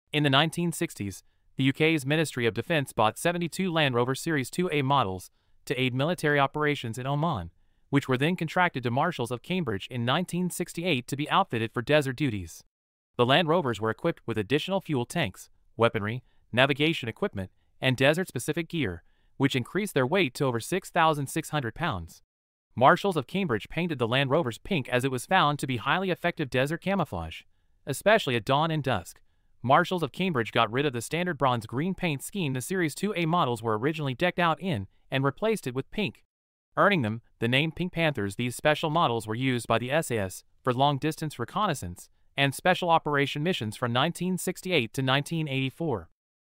Why The SAS Used Pink Jeeps For Desert Warfare
In the 1960s, the UK's Ministry of Defence purchased 72 Land Rover Series 2A models to support military operations in Oman. These vehicles were then modified for desert warfare by Marshalls of Cambridge, equipped with additional fuel tanks, weaponry, navigation equipment, and other specialized gear. To increase their effectiveness as desert camouflage, the Land Rovers were painted pink, which earned them the nickname "Pink Panthers" or "Pinkies."

In this video, we explore the history and specifications of these iconic Land Rovers, and how they were utilized by the SAS for long-distance reconnaissance and special operations missions from 1968 to 1984. We also delve into the reasons behind the choice of pink paint for desert camouflage, and the legacy of the Pink Panthers in military history. Join us as we uncover the fascinating story of these unique vehicles and their impact on modern warfare.